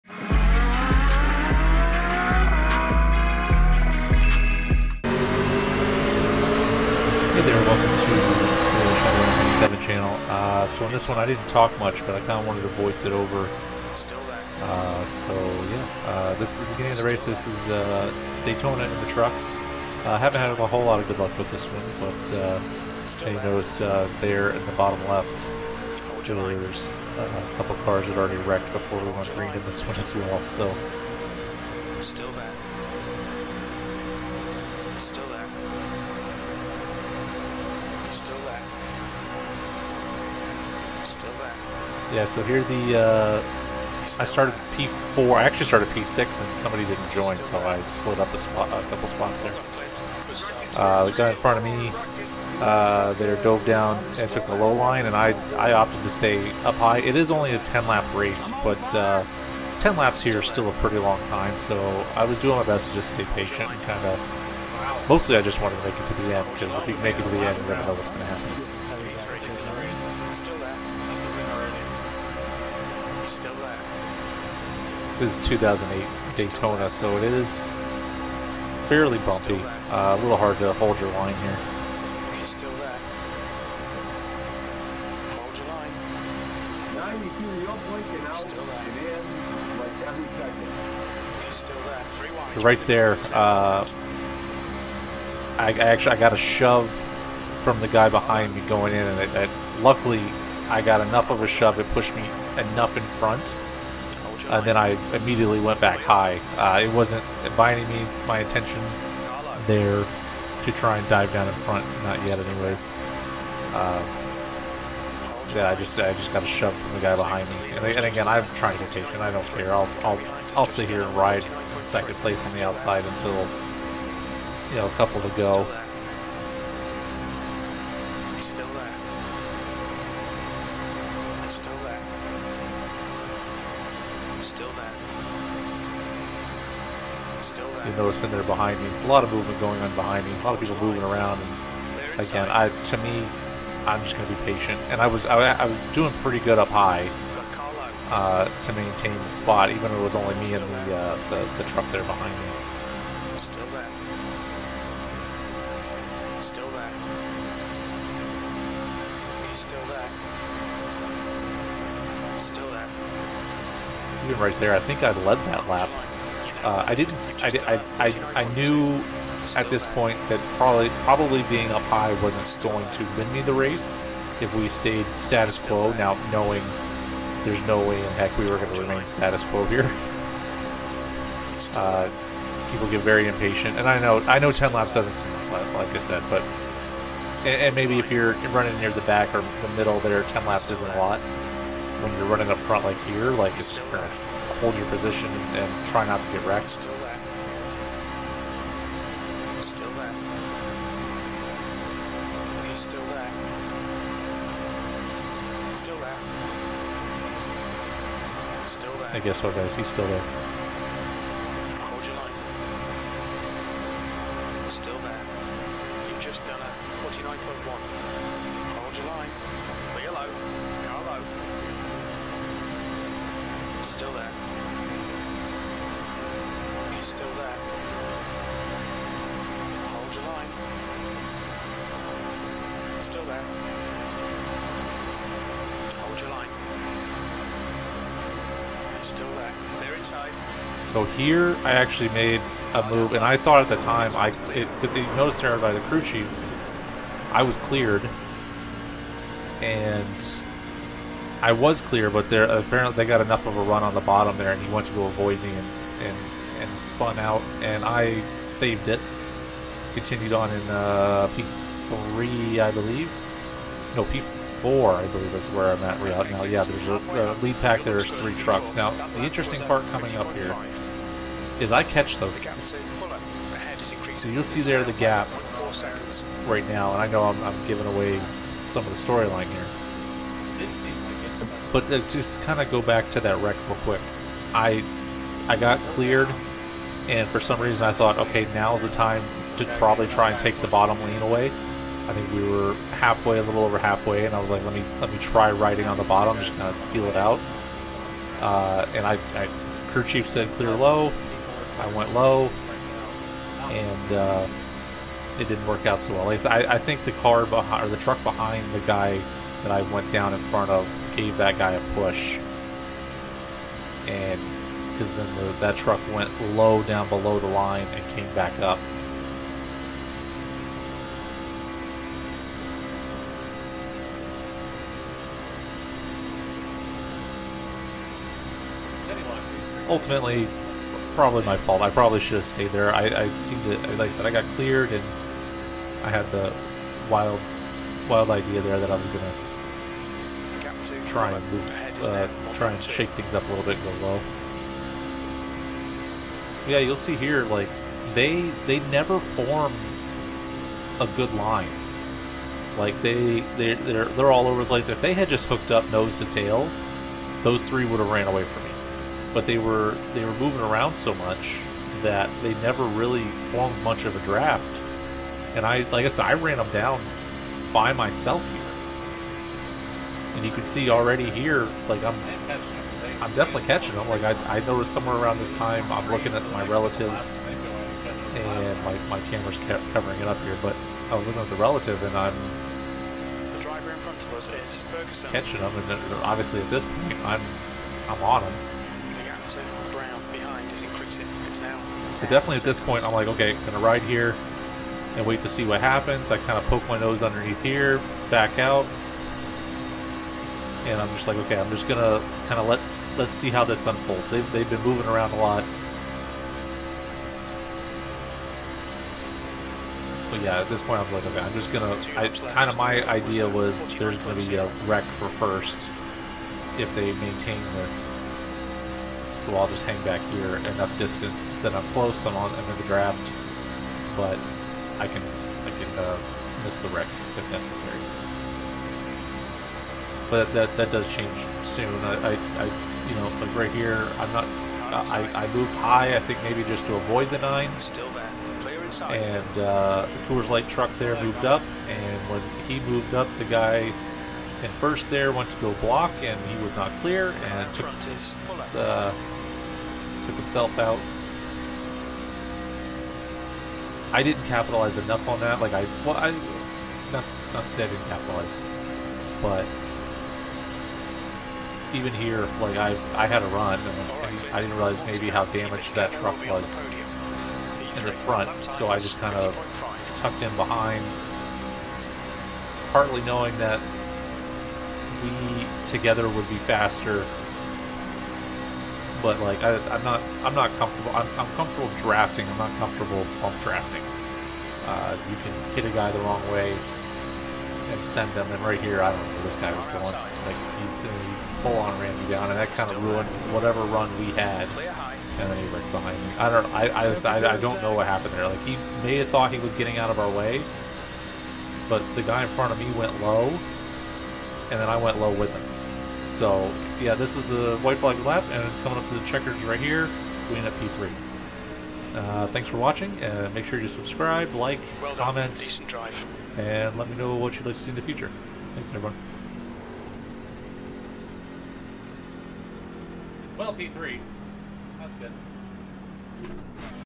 Lets give the trucks at Daytona another try...
Well, it has to go better this time right? At least this race doesn't end in a complete rant.